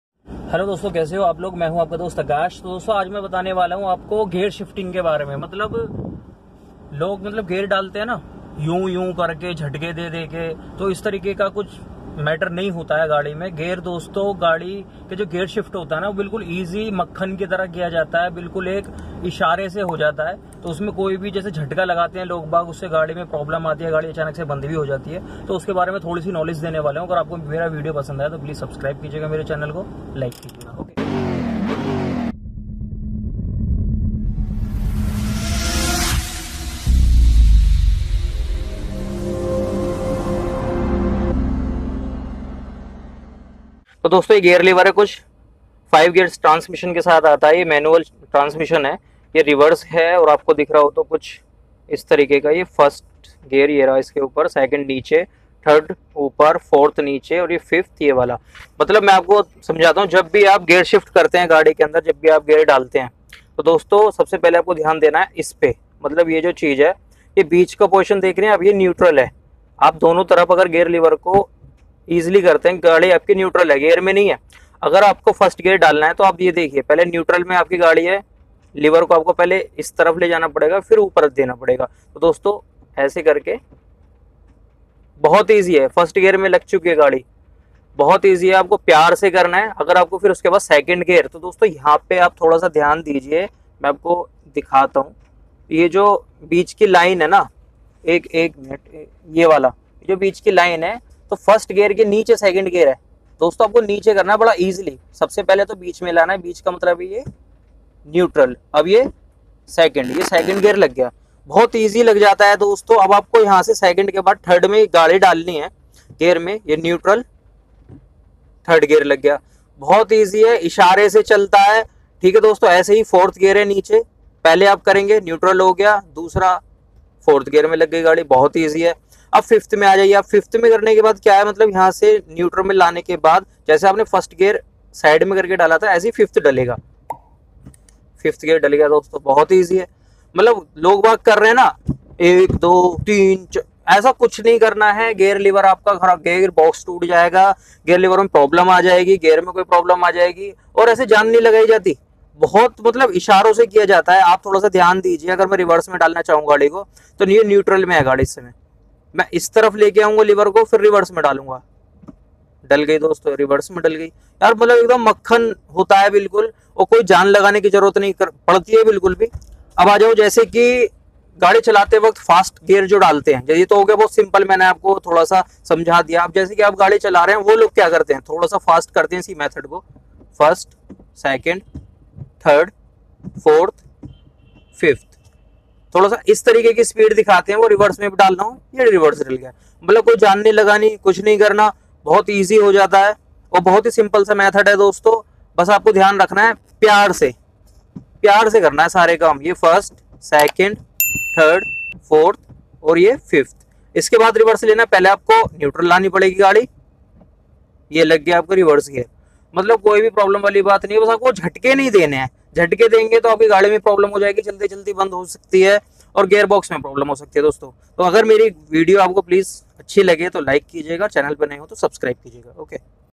How to change gear | Car Main Gear Dalna Sikho | The Vehiclez
How to change gear | Car Main Gear Dalna SikhoThe Vehiclez

iss video m apko btane vala hu ki car m gear kse dale bhot simple tarike s

aur mre dusre channel pr bhi pyar dikhaye
support kijiye